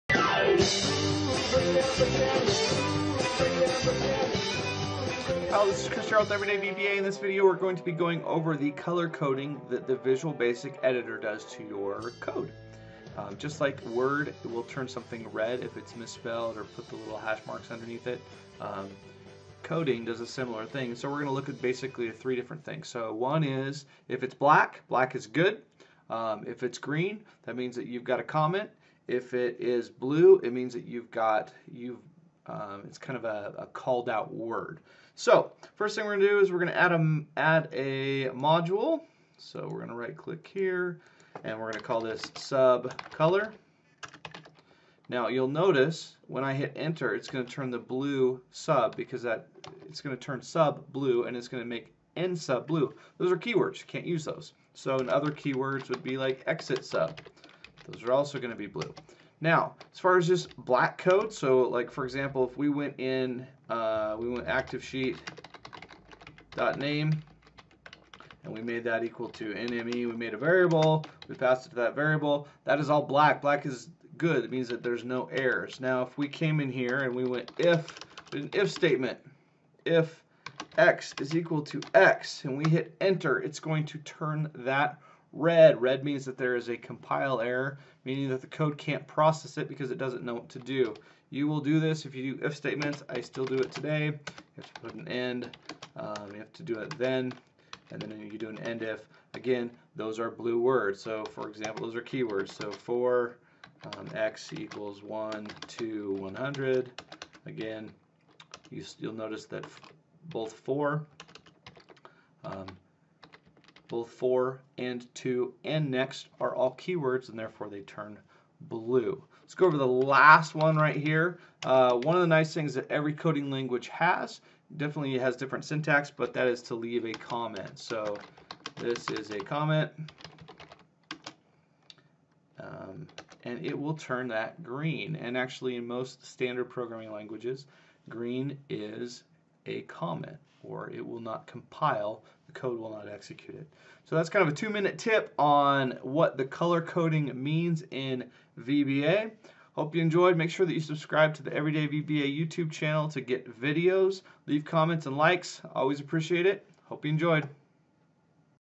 VBA Color Coding for Code, Keywords, Comments, and Errors
Ever wonder what the color coding is in Excel Macros (VBA).